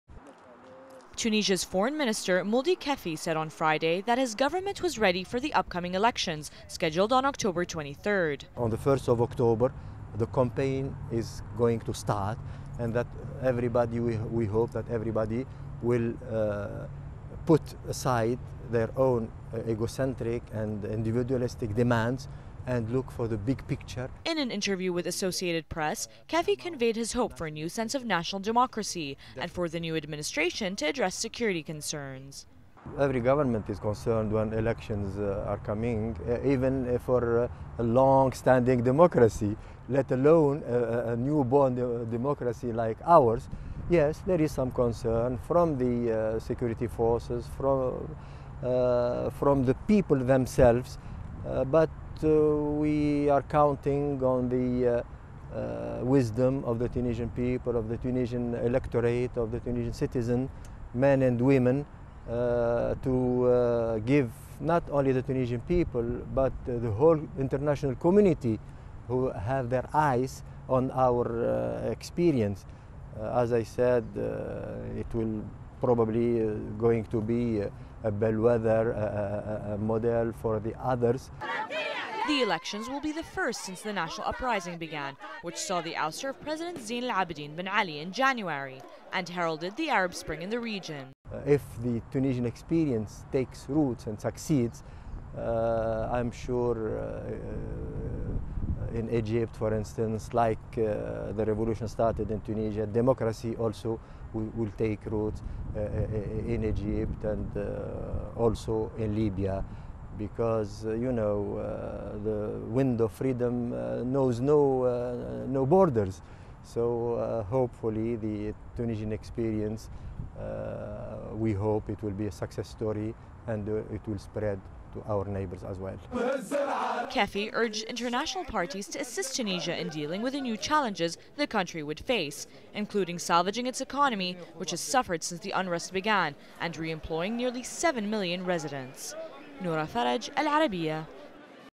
Tunisian FM on Upcoming Elections
Tunisia's Foreign Minister, Mouldi Kefi, said on Friday that his government was ready for the upcoming elections scheduled on October 23.

"On the first of October the campaign is going to start. We hope that everybody will put aside their own egocentric and individualistic demands and look at the big picture," he said.

In an interview with Associated Press, Kefi conveyed his hope for a new sense of national democracy and for the new administration to address security concerns.

"Every government is concerned when elections are coming. Even for long standing democracies let alone a new born democracy like ours. Yes, there is some concern from the security forces, from the people themselves, but we are counting on the wisdom of the Tunisian people, of the Tunisian electorate, of the Tunisian citizens, men and women, to give not only the Tunisian people but the whole international community who have their eyes on our experience. As I said it will probably going to be a bell weather model for the others," he said.

The elections will be the first since the national uprising began which saw the ouster of President Zine El Abidine Ben Ali, in January, and heralded the Arab Spring in the region.

"If the Tunisian experience takes root and succeeds, I'm sure in Egypt for instance, like the revolution started in Tunisia, democracy also will take root in Egypt and also in Libya. Because you know the wind of freedom knows no borders so hopefully the Tunisian experience - we hope - it will be a success story and it will spread to our neighbours as well," he said.

Kefi urged international parties to assist Tunisia in dealing with new challenges the country would face, including salvaging its economy which has suffered since the unrest began, and re-employing nearly seven million residents.

Speaker:

Mouldi Kefi - Tunisian FM